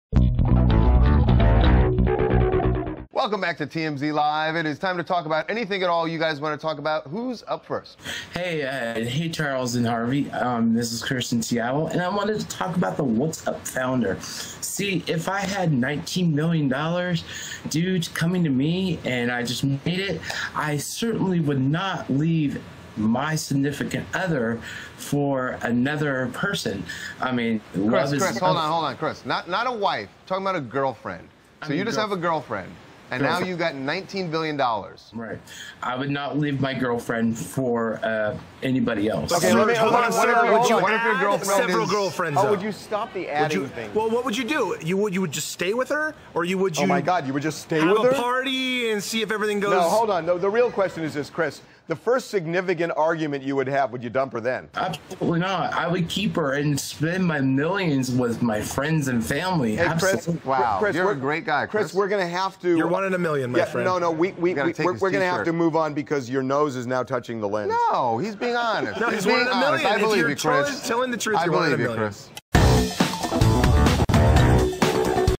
WhatsApp Co Founder Topic $19 Billion Upgrade Girl Freinds
i talked on tmzlive harvey levin and charles and i was like if i had $19 BILLION i would be spending my money on the one i love i would not need to buy a woman cause money Cant Buy Love.

This is WhatsApp co-founder Jan Koum in Spain with his girlfriend ... celebrating Facebook's $19 BILLION purchase of his company, and his 38th birthday.
So, the obvious question: when BILLIONAIRE Jan turns 39 ... will the same GF be in the picture?